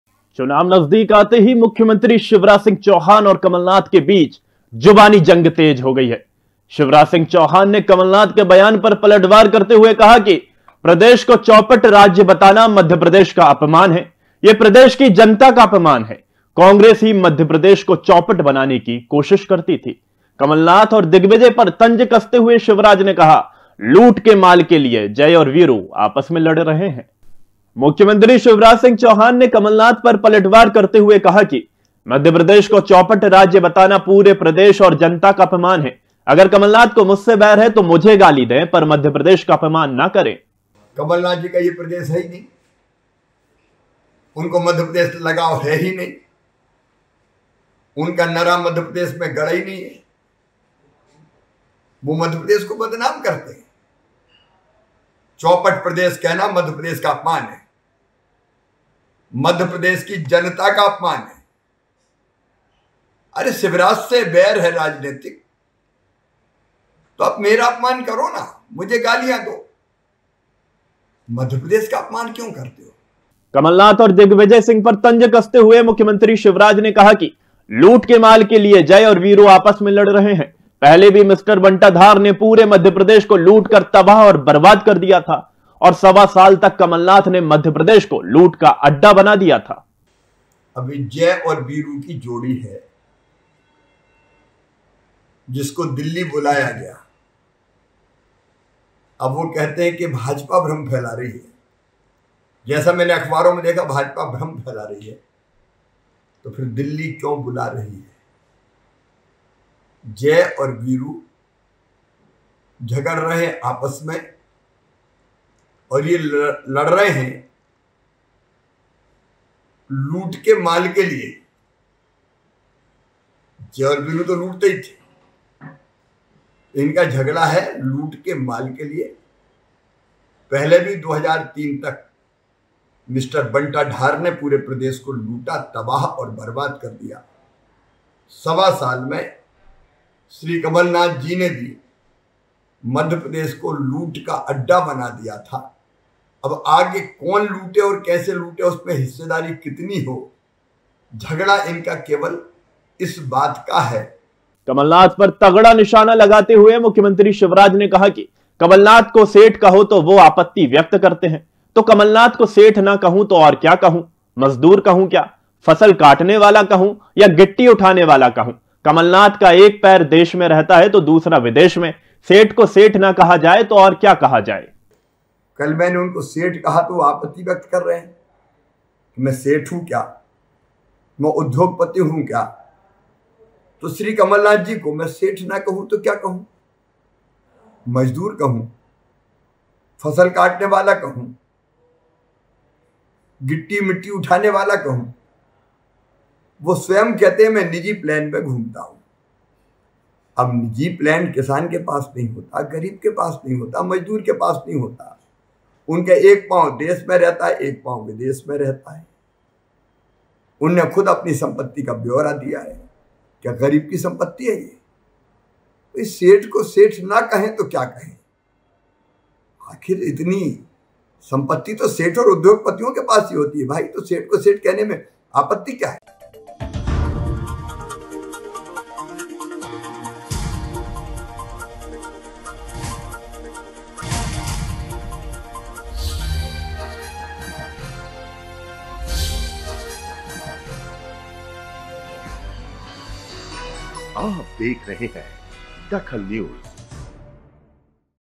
DAKHAL NEWS मुख्यमंत्री शिवराज का कमलनाथ पर पलटवार
मुख्यमंत्री शिवराज का कमलनाथ पर पलटवार
बोले कांग्रेस ने मध्यप्रदेश को चौपट बनाने की कोशिश की